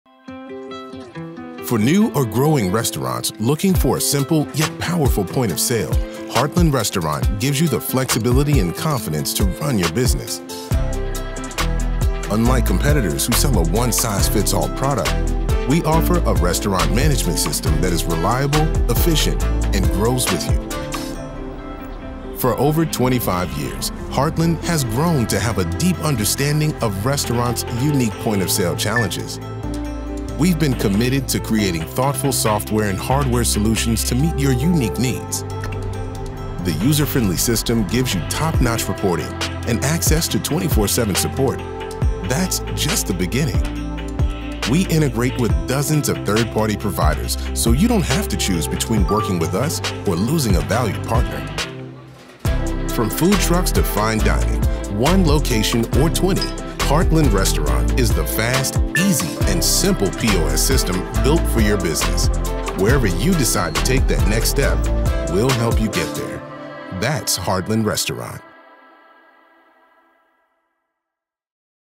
Heartland Restaurant Point of Sale | Introduction
In a world of constant daily interruptions due to payroll, payment and point of sale issues, someone had to find a way to make all of this work better, so you can get back to running your business.

With the right tools, the human touch and the strong foundation, Heartland is how business owners can finally get one solution for everything to run and grow their business better.

It's time to make every day work better. Visit Heartland.us to begin.